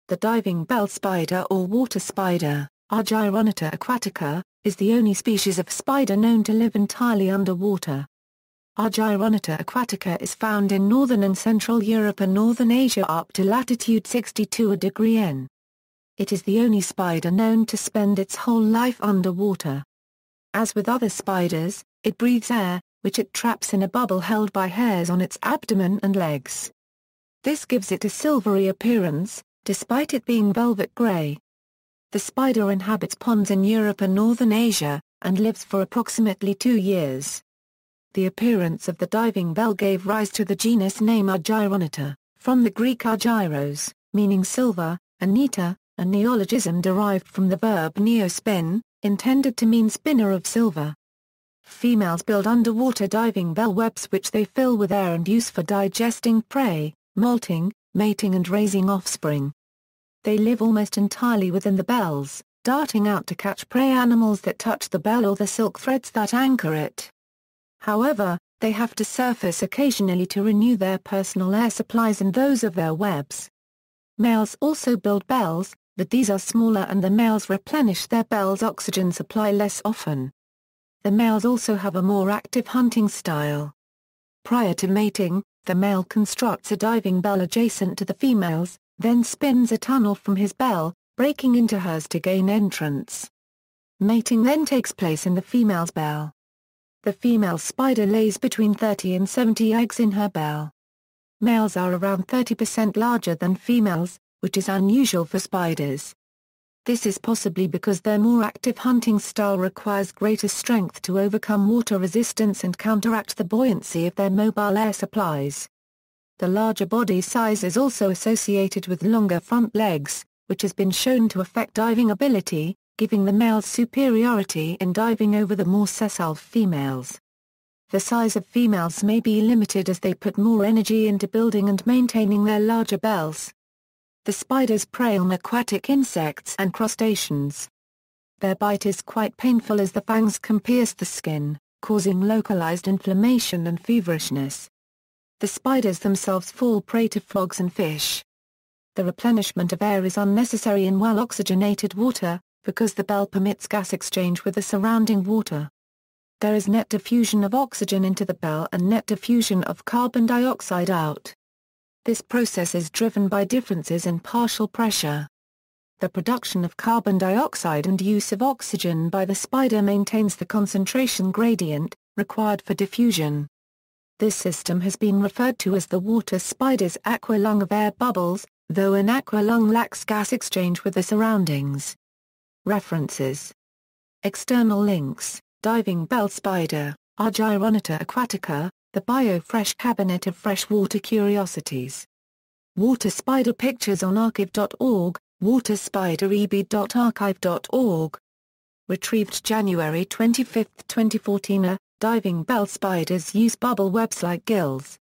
Diving bell spider
The diving bell spider or water spider, Argyroneta aquatica, is the only species of spider known to live entirely under water.
Argyroneta aquatica is found in northern and central Europe and northern Asia up to latitude 62°N. It is the only spider known to spend its whole life under water. As with other spiders, it breathes air, which it traps in a bubble held by hairs on its abdomen and legs. This gives it a silvery appearance, despite it being velvet-grey. The spider inhabits ponds in Europe and northern Asia, and lives for approximately two years. The appearance of the diving bell gave rise to the genus name Argyroneta, from the Greek "argyros" (ἄργυρος), meaning "silver", and "neta", a neologism (perhaps for *νητής) derived from the verb "neo" (νέω) "spin", intended to mean "spinner of silver".